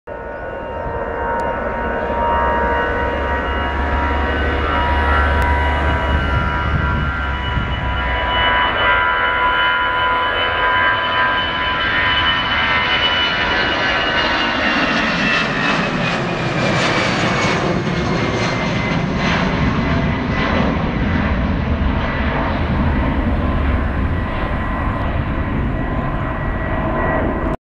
TAP Airbus A-319 take-off from Porto
Airbus A-319 from Air Portugal TAP taking-off Porto airport's runway 35.